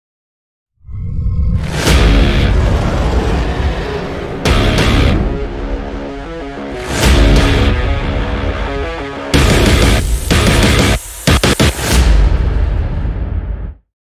Project Chaos Esports Trailer
Welcome to Project Chaos Esports
Contributing to the online video game Esports by creating and managing tournaments worldwide to reward top players and provide entertainment for all game lovers.